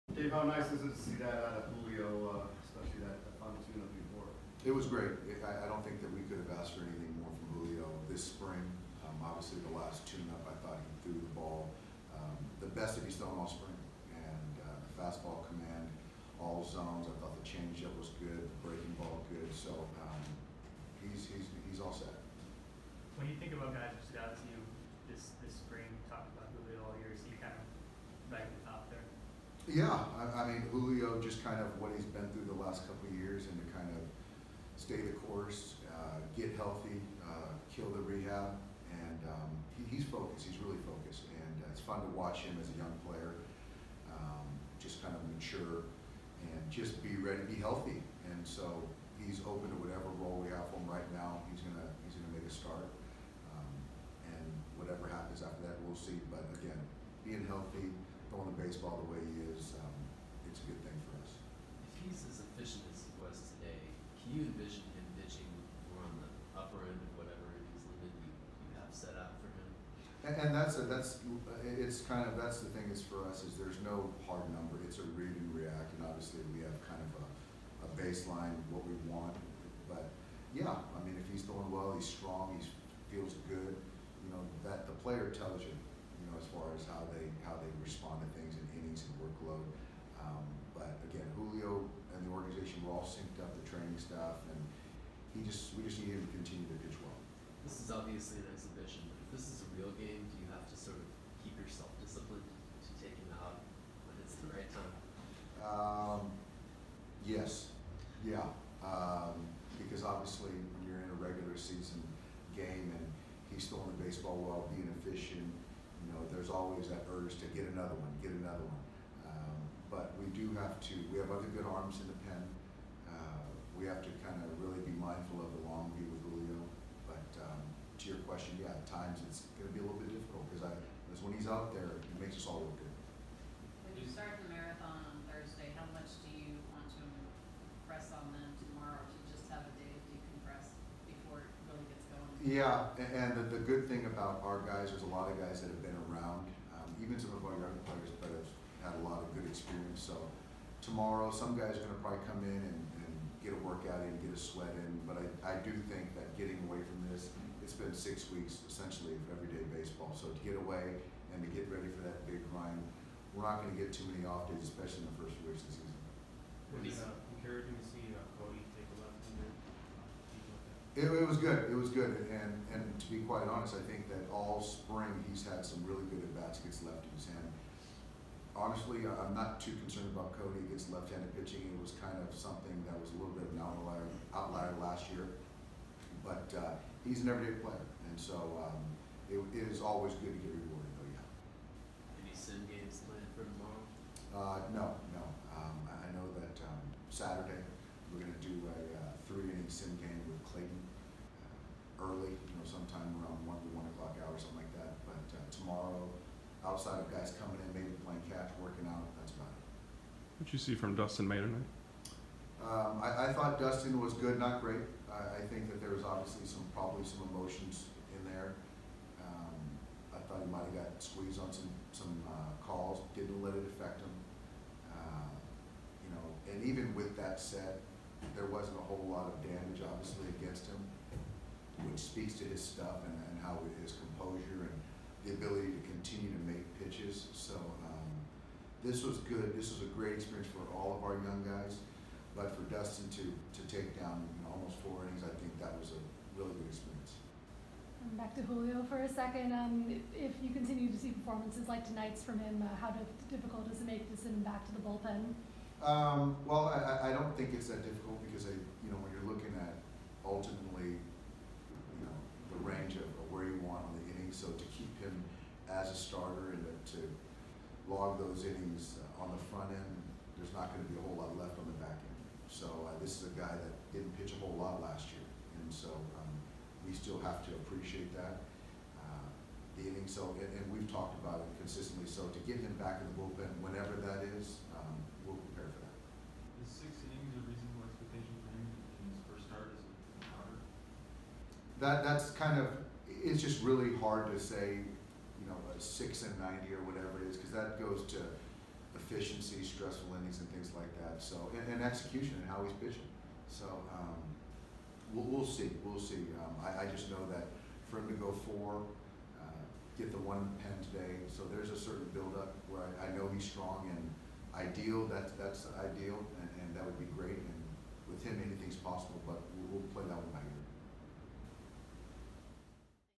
Dodgers postgame: Dave Roberts mindful of big picture with Julio Urias
Los Angeles Dodgers manager Dave Roberts discussed the team's focus and handling of Julio Urias and his innings limit, plus Dustin May pitching at Dodger Stadium for the first time.